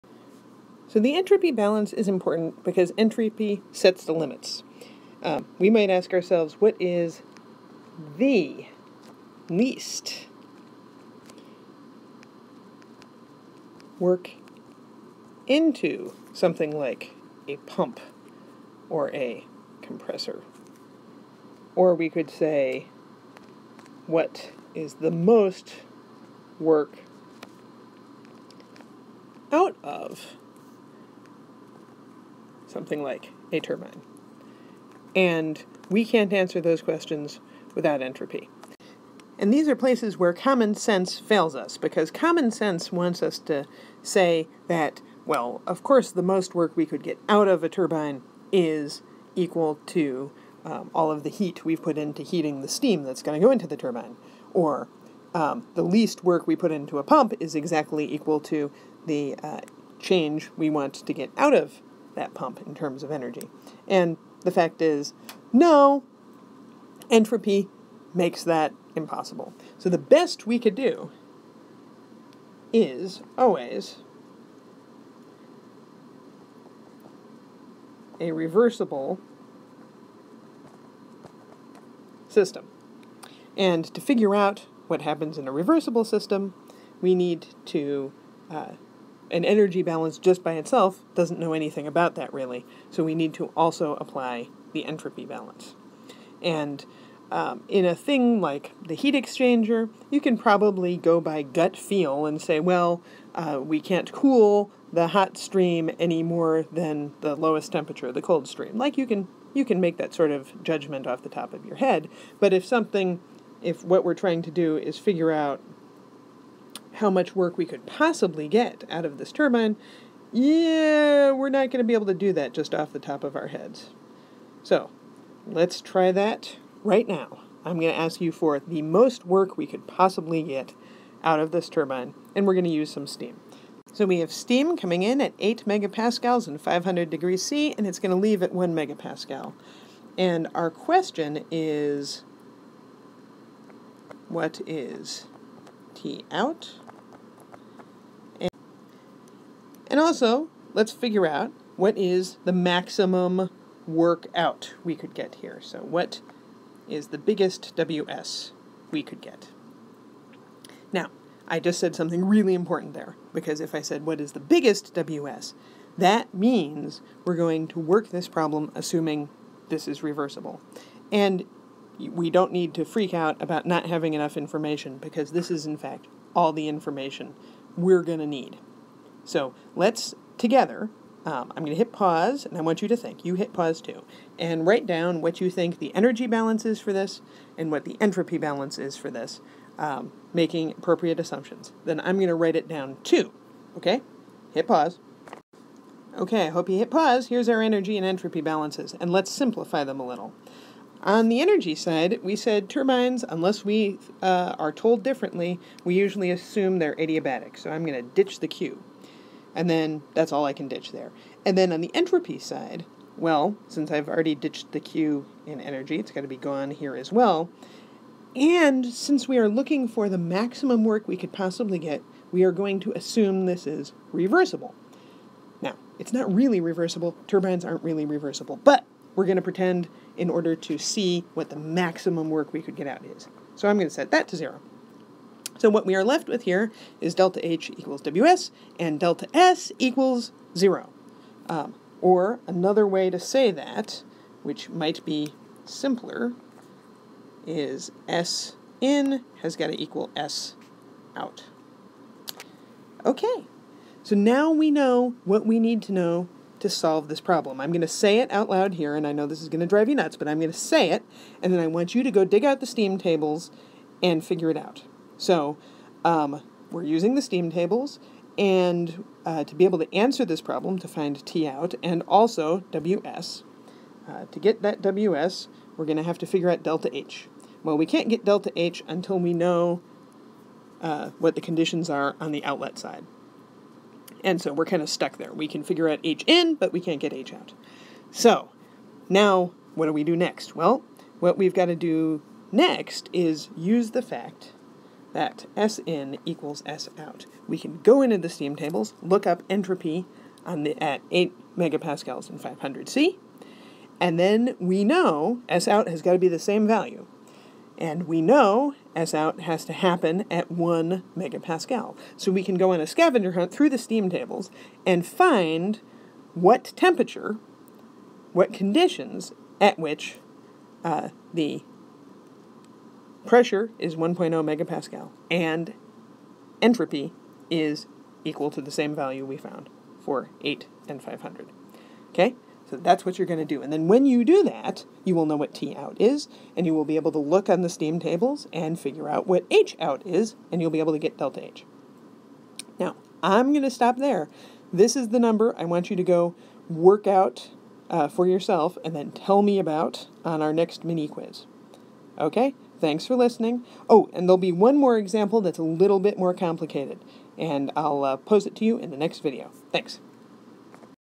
Turbine example 1 2015
This is a simple example of how you apply energy and entropy balances to determine the maximum possible work out of a steam turbine.